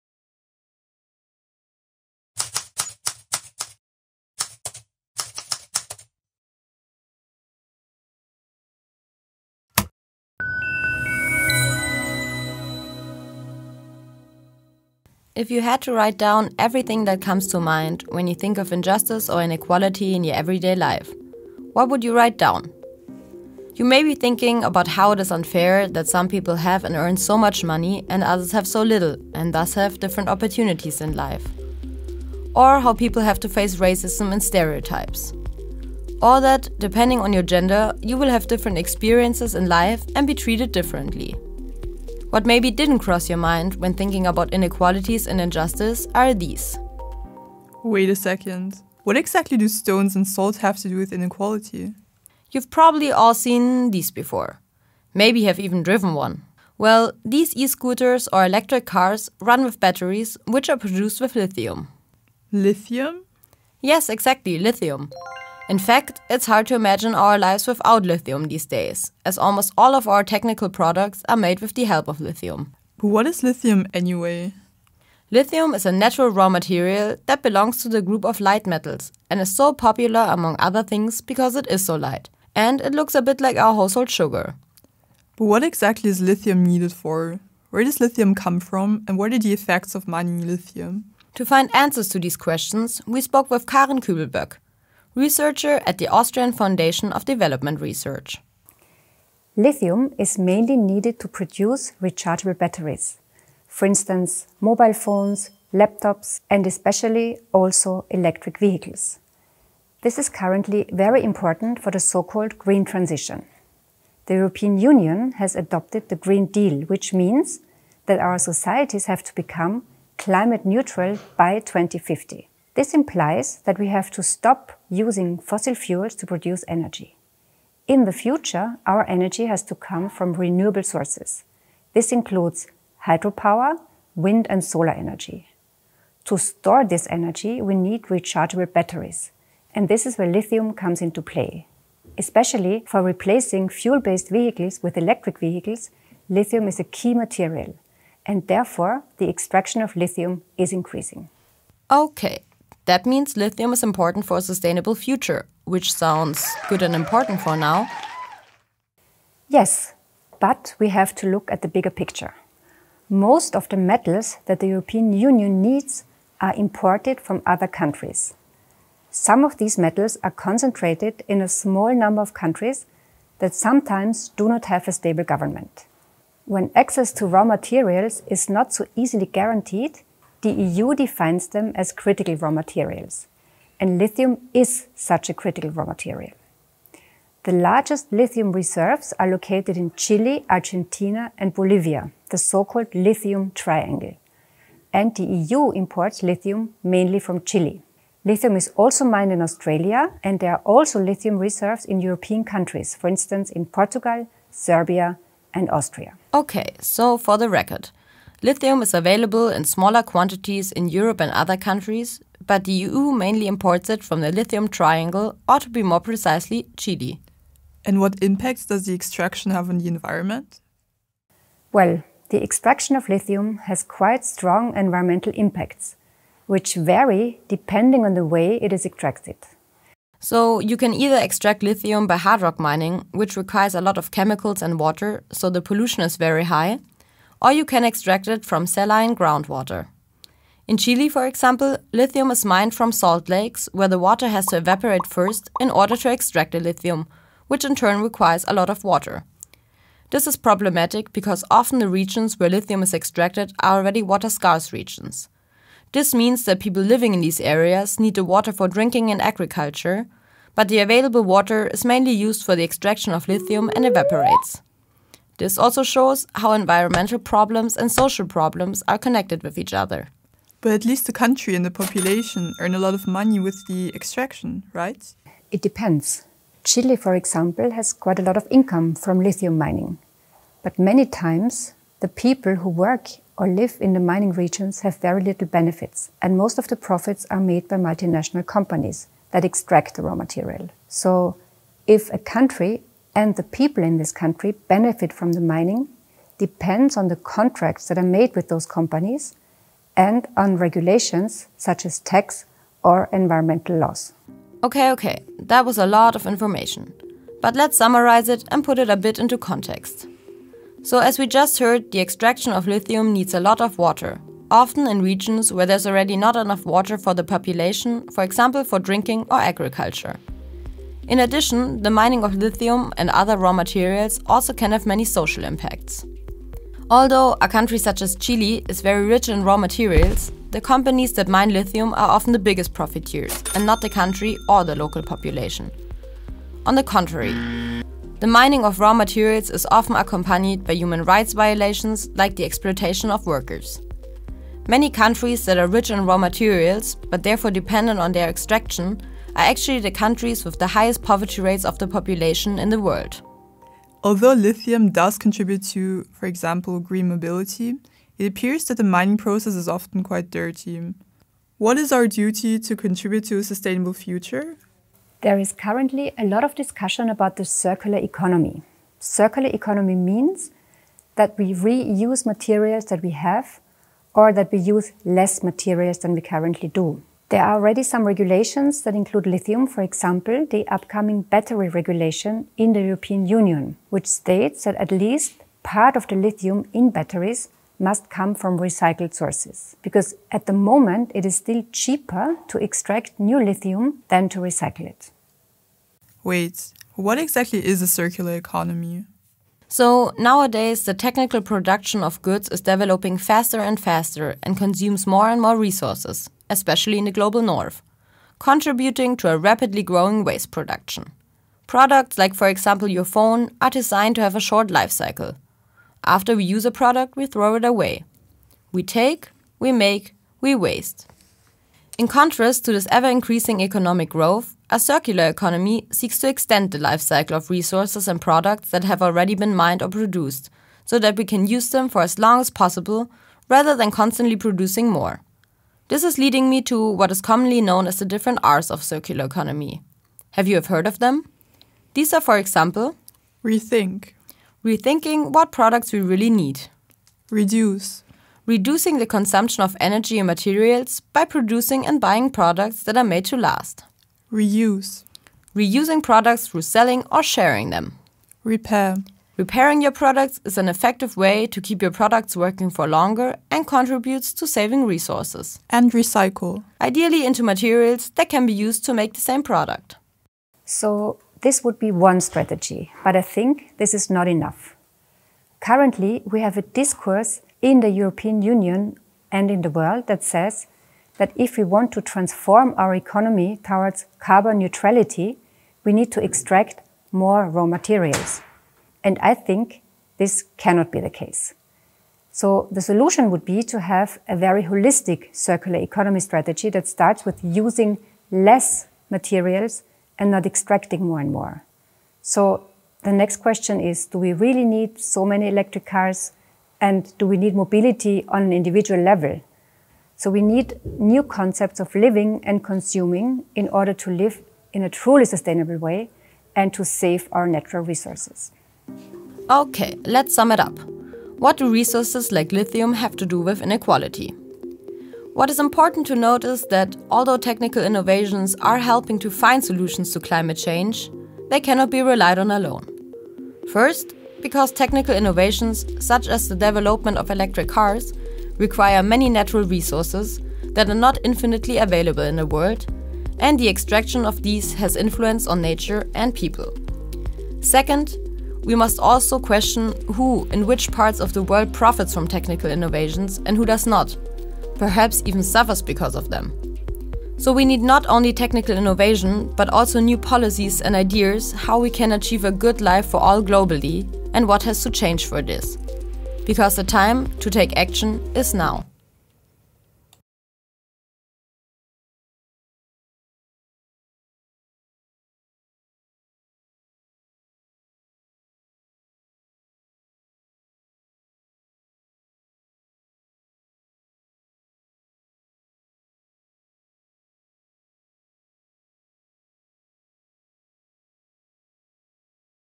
Facing (in)equalities: Lithium-extraction & global impacts of e-mobility (GER, ENG, HUN & POL subs)
This video has subtitles in English, German, Polish and Hungarian.

Green technology is considered a key strategy for addressing the climate crisis: Can the energy transition succeed through these innovations? - And if so, at what price? What are the global effects of mining the raw materials required for this? And can we simply make our way of life green through technological advances without changing our behaviour?
In the video, we deal with these questions using the example of e-mobility and the necessary raw material lithium. We talk to expert Karin Küblböck, travel to resource-rich countries in the Global South and get to know the alternative concept of the circular economy.

In grüne Technologien werden große Hoffnungen bei der Bekämpfung des Klimawandels gesetzt: Kann durch diese Innovationen die Energiewende gelingen – und wenn ja, zu welchem Preis? Welche globalen Auswirkungen hat der Abbau der dafür notwendigen Rohstoffe? Und können wir unsere Lebensweise einfach durch technologischen Fortschritt grün machen, ohne unser Verhalten ändern zu müssen?
Im Video behandeln wir diese Fragen am Beispiel von Elektromobilität und dem dafür notwendigen Rohstoff Lithium. Wir sprechen mit Expertin Karin Küblböck, reisen in rohstoffreiche Länder des Globalen Südens und lernen das alternative Konzept der Kreislaufwirtschaft kennen.

// This video is part of a learning program developed in Erasmus+ project FutureSIM: Making Europe’s Future Sustainable co-funded by the European Union.